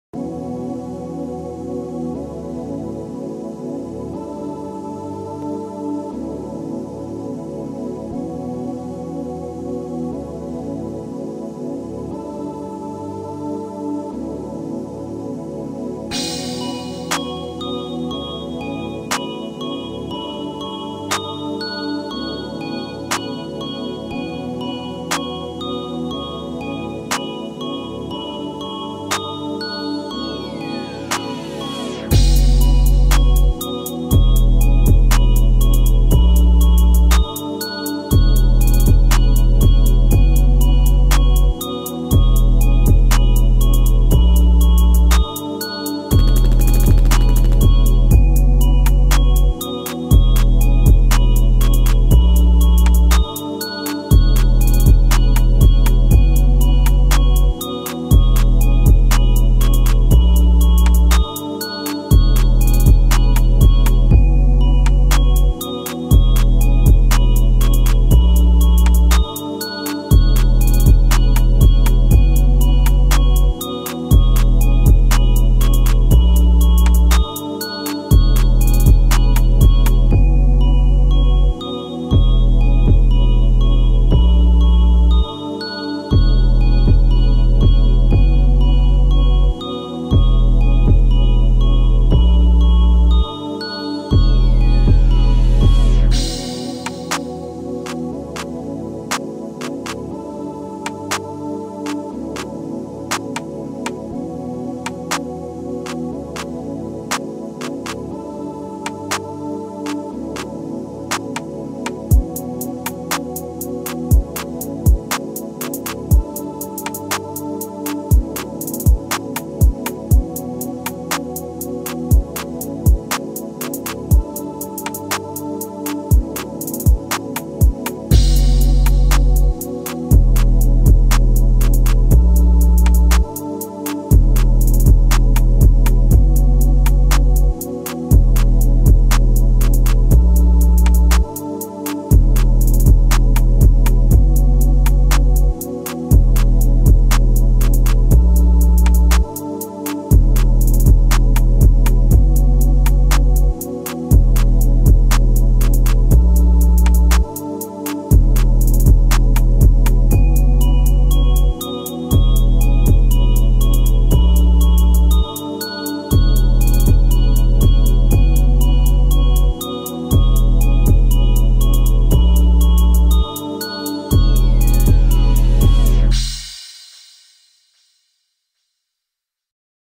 (FREE) ICYTWAT/ Divine Council/ Nolan Wrld type beat
i literally 4got 2 upload dis beat like months ago lmfaoo but it was before i knew how 2 mix but it still sounds good 2 me Lol